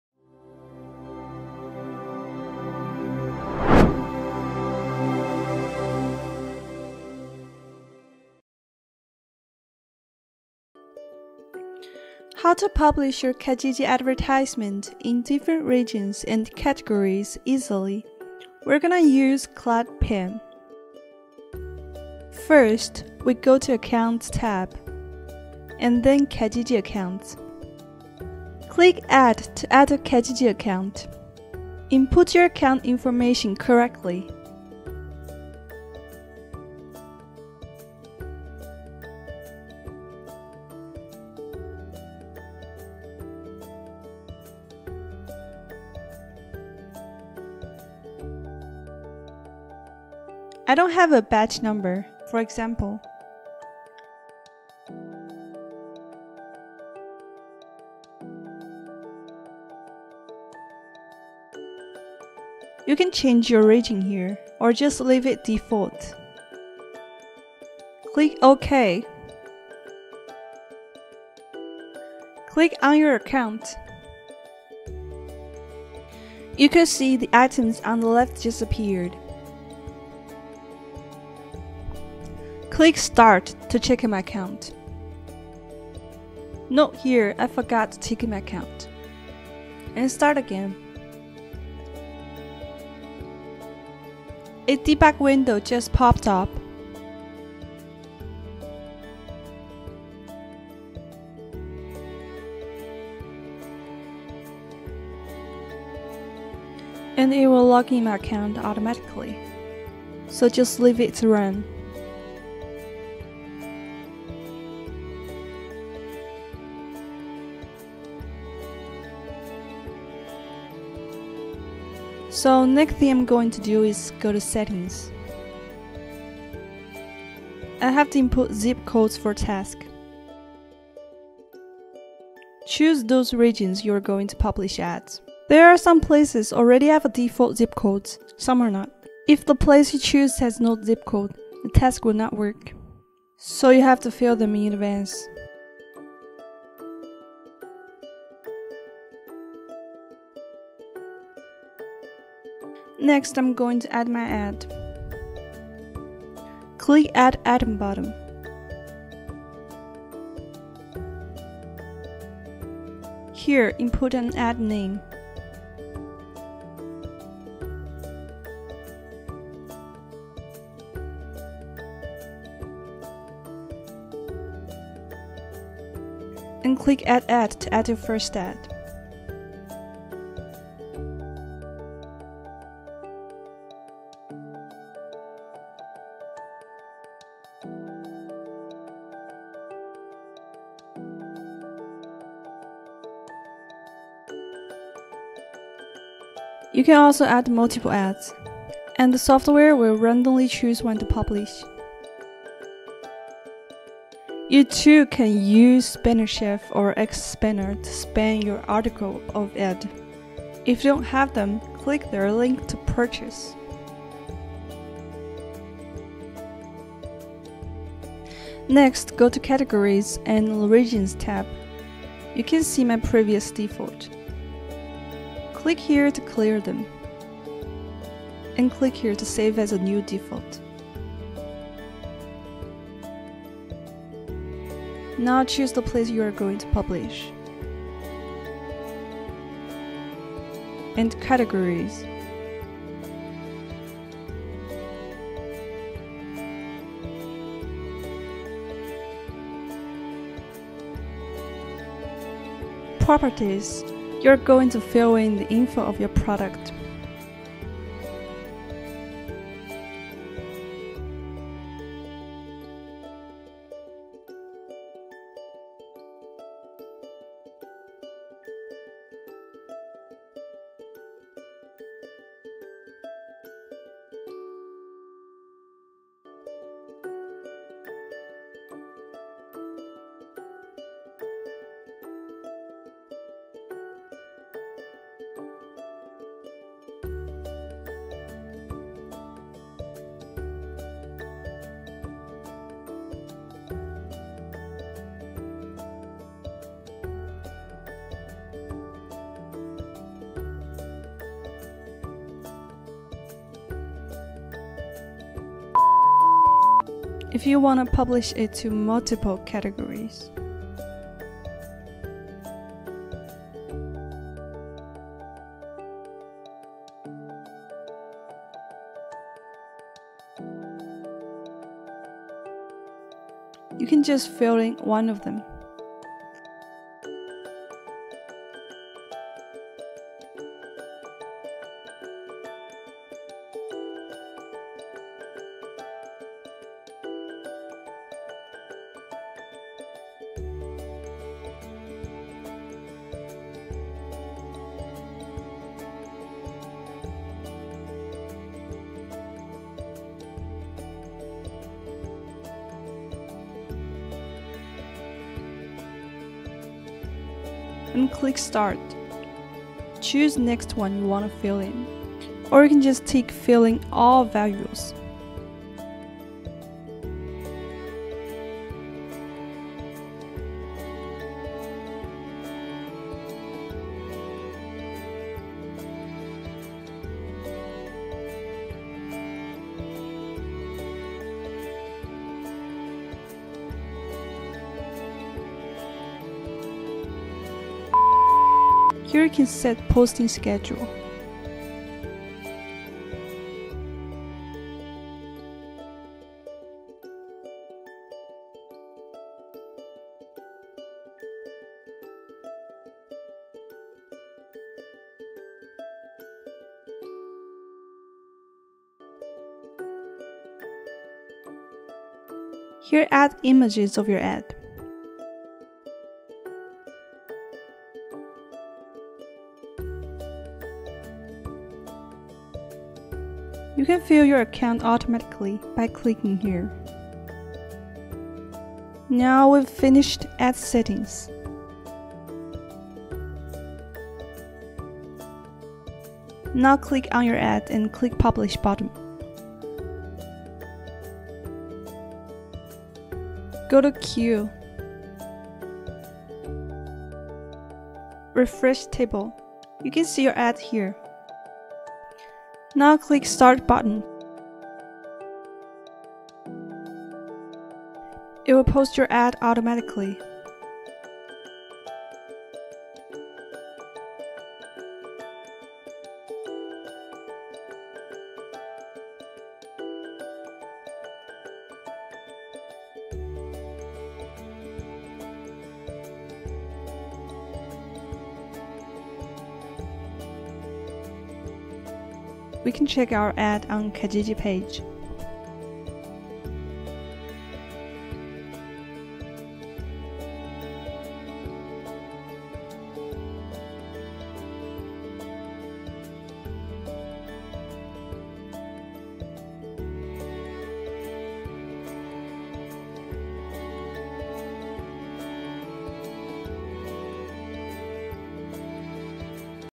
CladPin Tutorial| Kijiji Bot|Auto post multiple Ads on Kijiji in different Cities
Cladpin is Developed by WhiteHatBox,

Now, you too can use free classified ads to sell your products, attract potential clients, promote your brands, and more!

Best part: Everything from creating an account
selecting the geo-location
posting your ads
even creating multiple ads could be done by doing a few simple clicks. No hassle guaranteed!